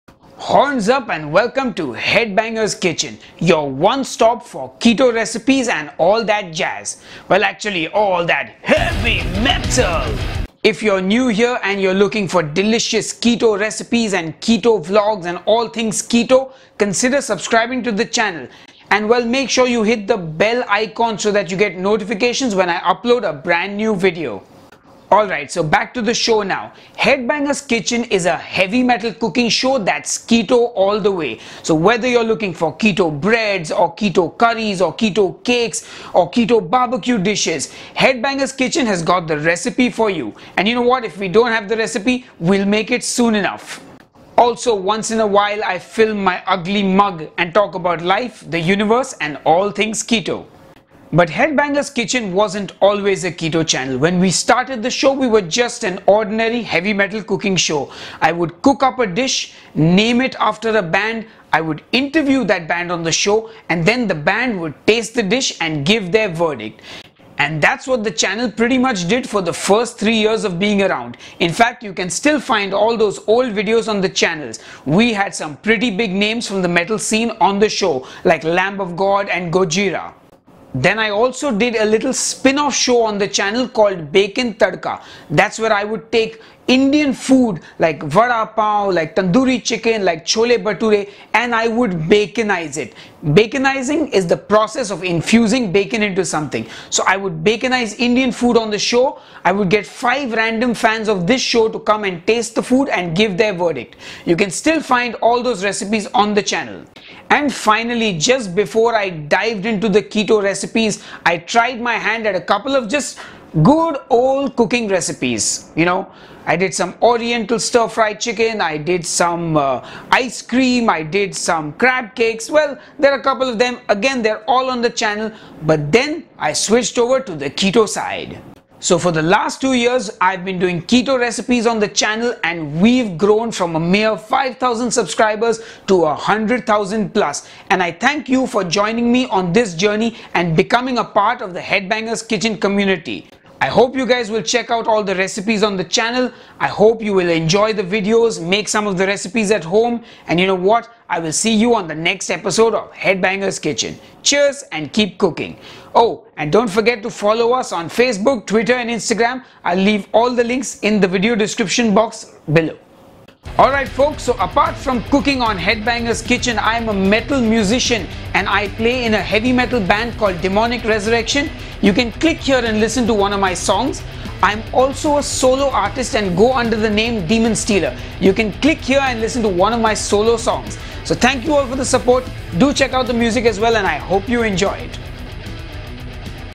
Welcome to Headbanger's Kitchen - Keto All The Way! (Channel Trailer)
Welcome to Headbanger's Kitchen - Keto All The Way. Your one stop for everything related to the keto diet or keto lifestyle. We do recipes for just about everything keto, whether you want a keto bread recipe or a keto dessert or even a keto curry. You've come to the right place.

Both stores ship worldwide, just shipping costs vary.

Thank you all for the support and I look forward to sharing this keto journey with you.

Cheers & Keep Cooking!
Demonstealer